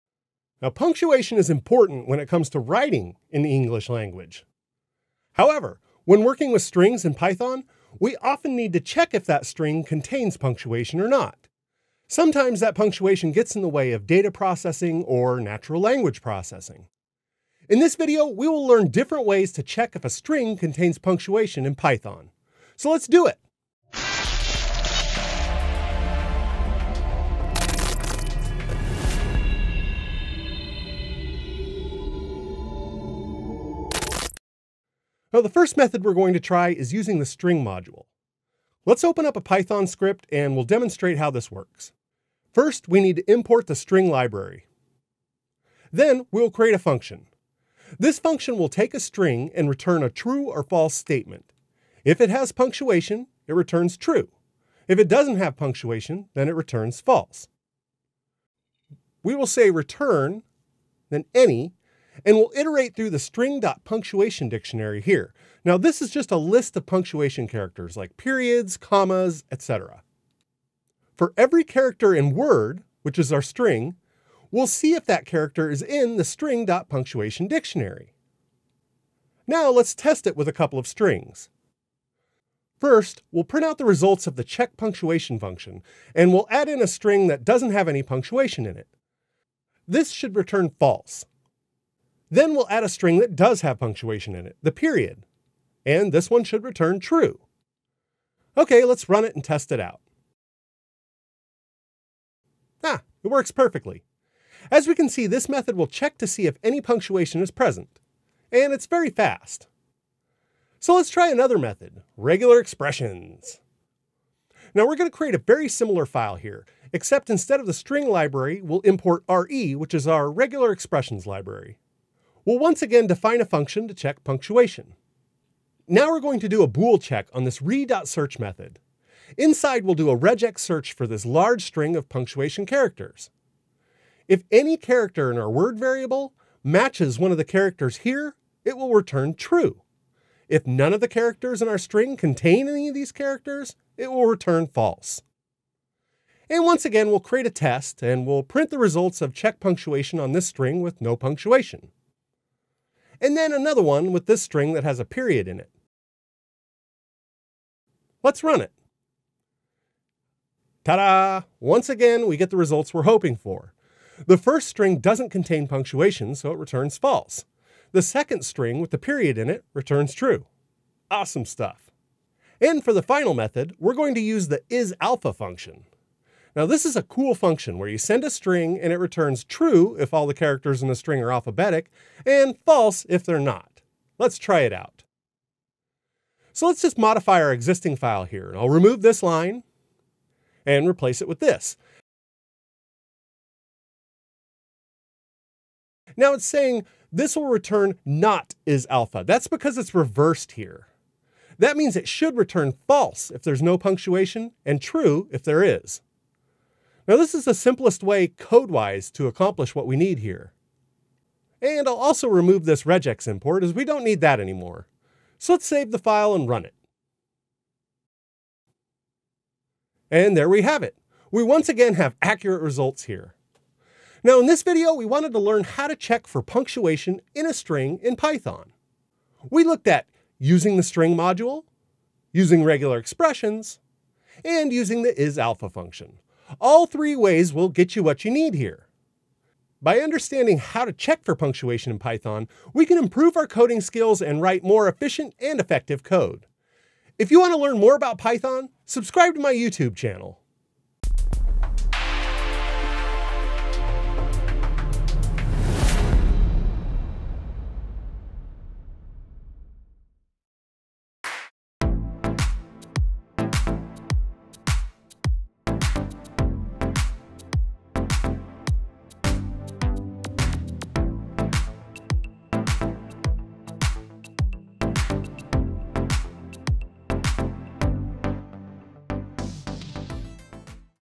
How to check if a string contains punctuation in Python
Let's explore three different ways to check if a string contains punctuation in Python. With code examples.

It's important to check for punctuation in a string. This can be helpful for cleaning up data, and in Natural Language Processing. Sometimes you just need the words, not the punctuation.